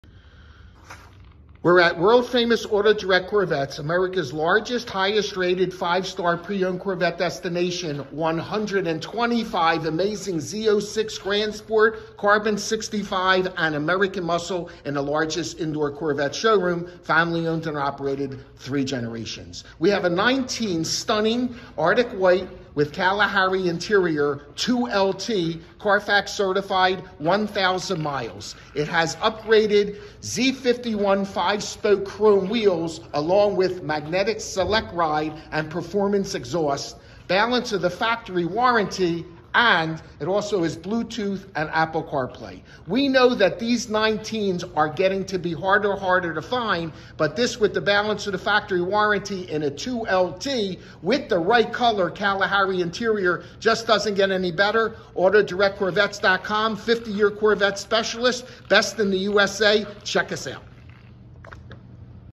2019 Corvette Stingray Coupe
Largest Indoor Corvette Showroom🇺🇸

125 Corvettes & Exotics

2019 Corvette stingray 2LT coupe Carfax certified only 1K miles in pristine condition!